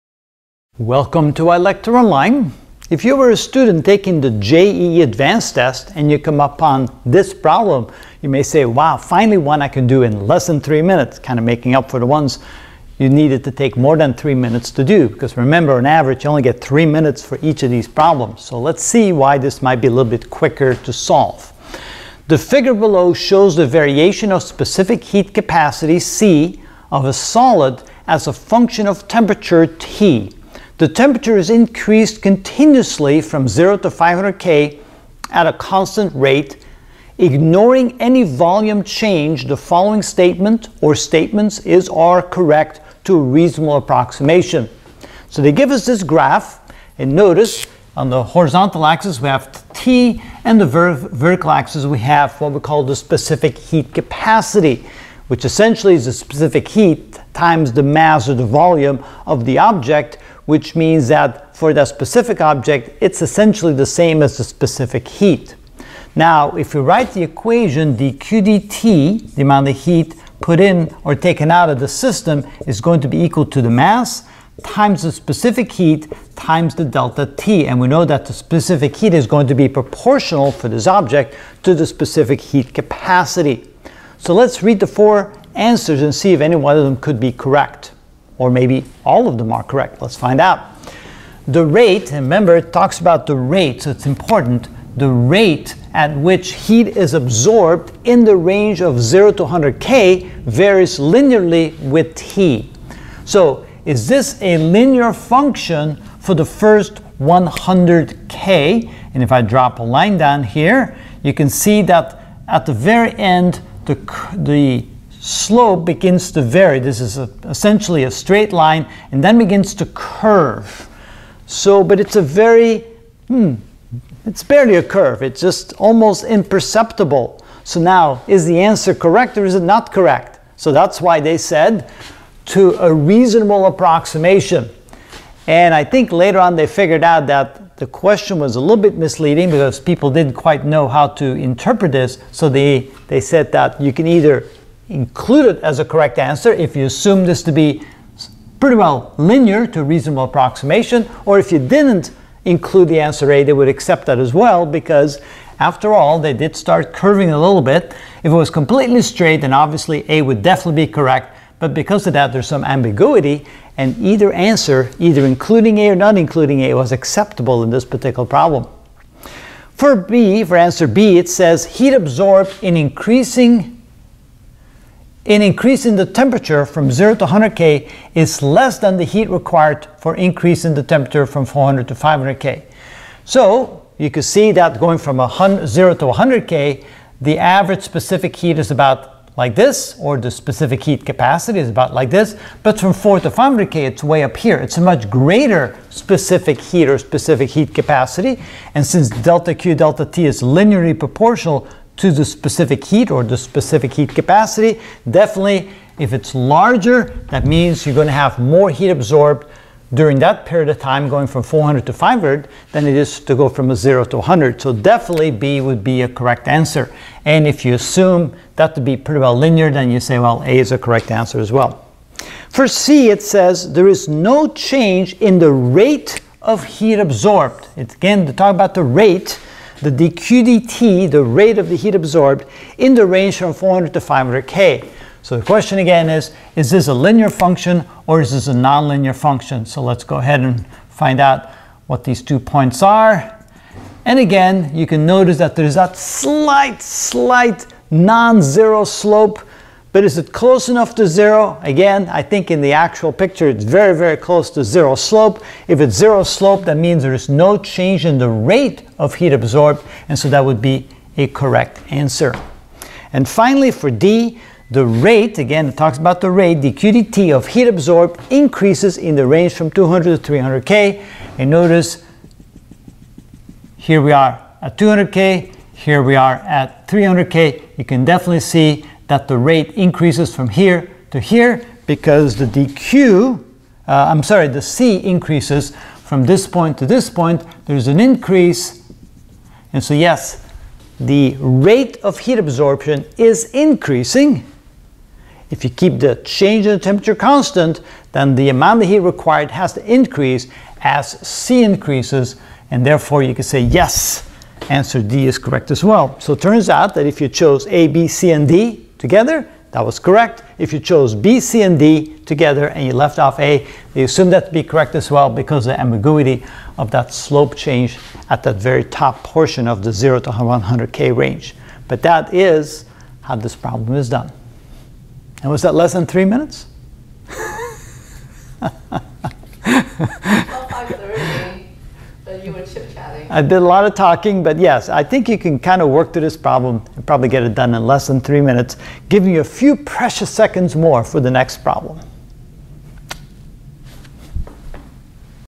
JEE Advanced Physics 2013 Paper 2 #3 (#10) Specific Heat
The figure below shows the variation of specific heat capacity (C) of a solid as a function of temperature (T). The temperature is increased continuously from 0 to 500K at a constant rate ignoring any volume change, the following statement(s) is (are) correct to a reasonable approximation:
A) The rate at which heat is absorbed in the range of 0-100K varies linearly with T.
B) Heat absorbed in increasing the T from 0-100K is less than the heat required from increasing T from 400-500K.
C) There is no change in the rate of heat absorbed in the range 400-500K.
D) The rate of heat absorbed increases in the range 200-300K.

Next video in this series can be seen at: